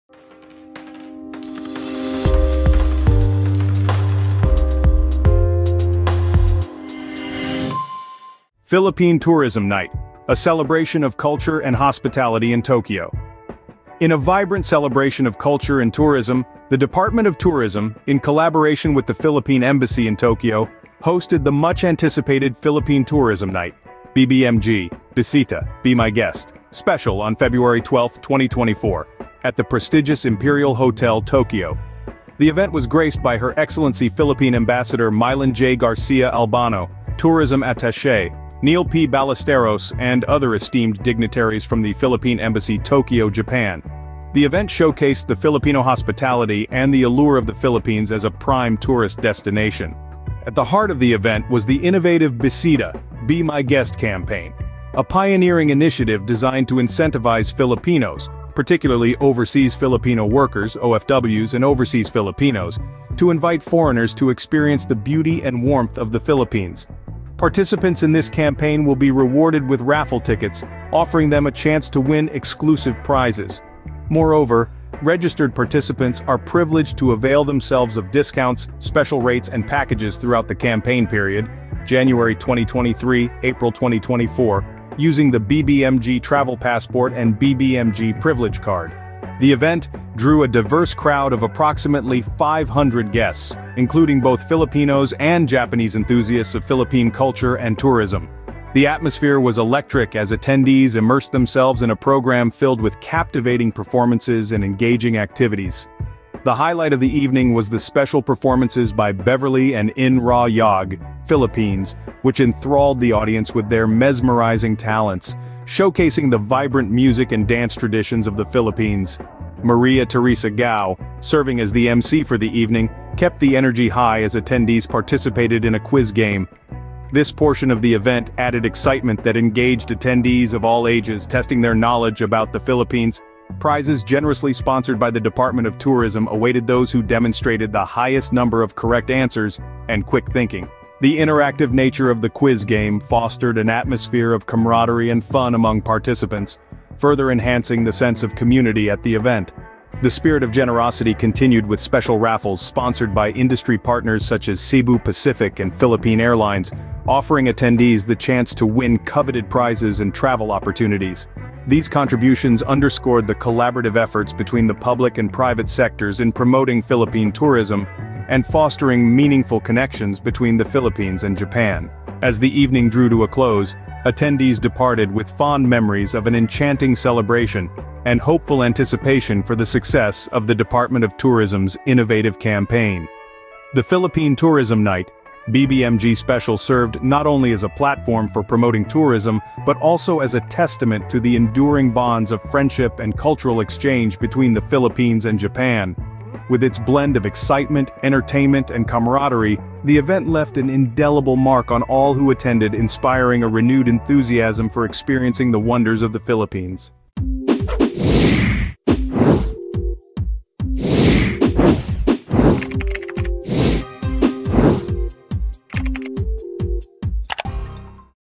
Philippine Tourism Night: A Celebration of Culture and Hospitality in Tokyo
Highlights from the Department of Tourism's innovative campaign and exciting event in collaboration with the Philippine Embassy Tokyo.

For more interesting and entertaining stories about Japan and the Philippines,